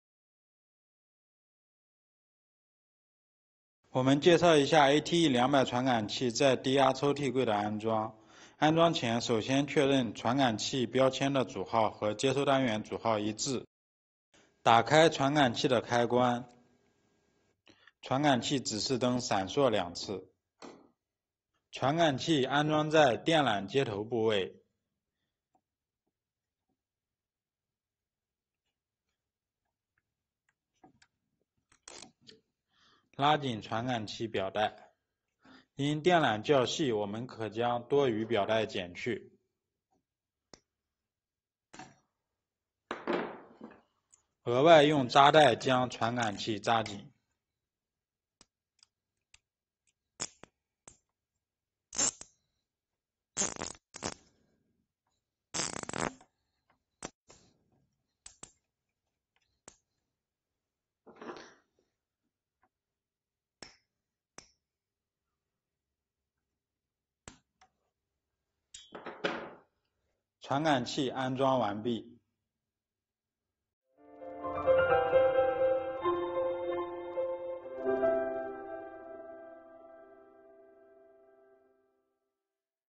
安科瑞电气 | 表带式ATE200无线节点测温传感器在低压抽屉的安装
ATE系列无线测温传感器根据《NB/T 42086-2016无线测温装置技术要求》开发，可用于3~35kV户内开关柜，包括中置柜、手车柜、固定柜、环网柜等多种开关柜；也可用于0.4kV低压柜，包括固定柜，抽屉柜等。无线测温传感器可安装于柜内任何发热点上，利用无线数据传输技术实时发送监测数据，再通过接收装置将温度数据传输到显示装置或者远程监控系统。

ATE200表带式无线测温传感器适用于断路器动触头、静触头、电缆接头、母排等处。表带式无线温度传感器结构说明：①无线温度传感器主体，测温探头在背面；②表带及锁扣；③电池开关。将传感器主体固定在安装位置，将表带绕过安装母排或者触头穿过表带，表带头收紧， 将表带尾用锁扣锁紧。表带过长可以剪掉多余部分，表带过短可以联系我司增订。安装前打开电池开关，电源指示灯闪烁两次。